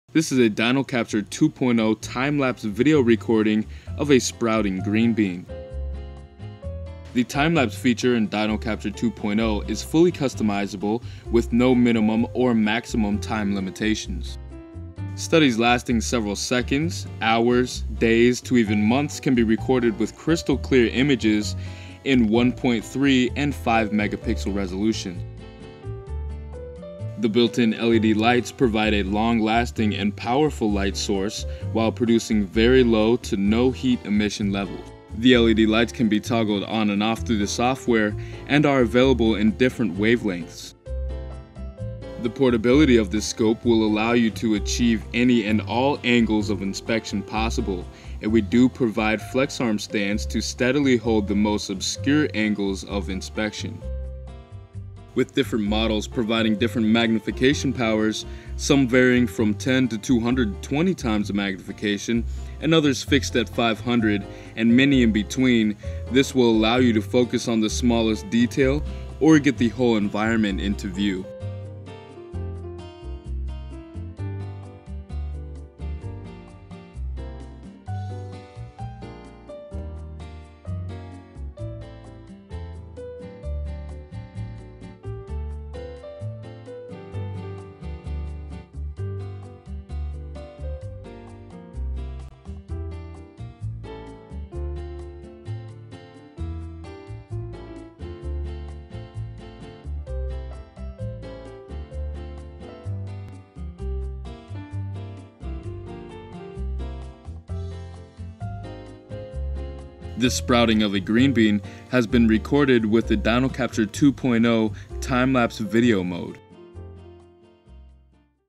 Time Lapse Sprouting Green Bean
Time Lapse Sprouting Green Bean captured with Dino-Lite Microscope time lapse video mode.